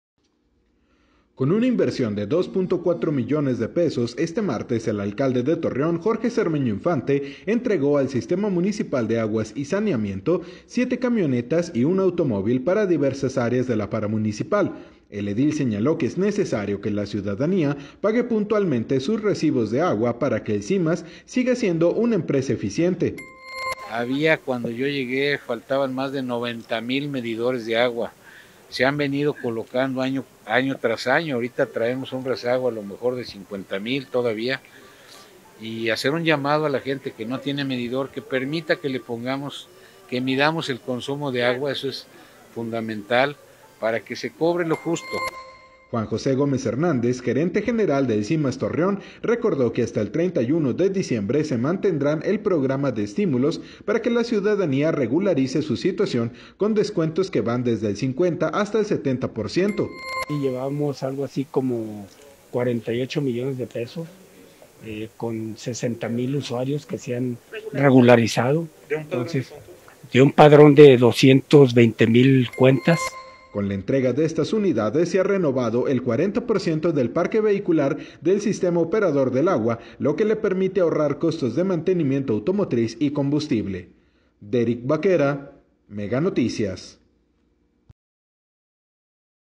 Entregan ocho vehículos al Simas; alcalde pide a usuarios regularizarse.
→ En lo que va de la administración se ha renovado el 40 por ciento del parque vehicular.